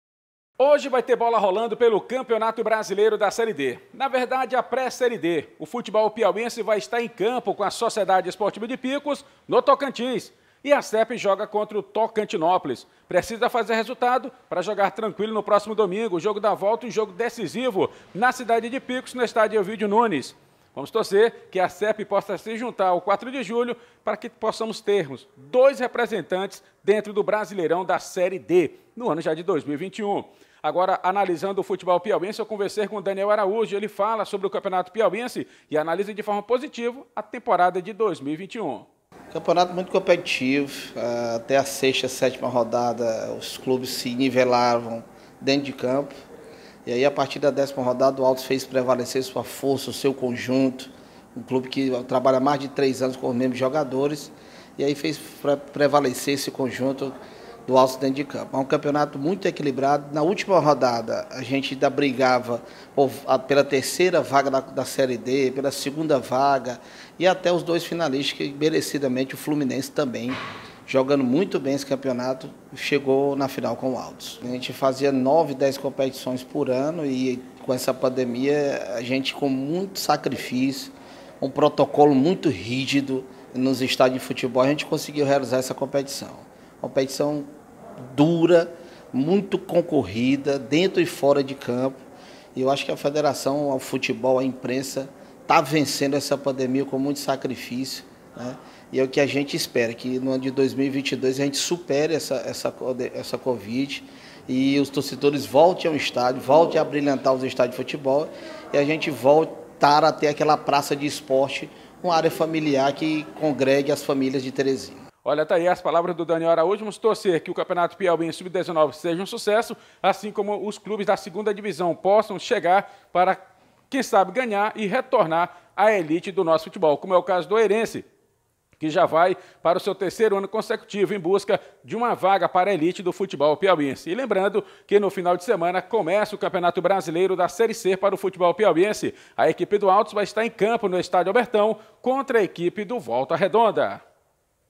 Daniel Araújo analisa o campeonato piauiense de 2021e destaca as próximas competições
Federação de futebol do Piauí irá realizar série B e Sub-19 no segundo semestre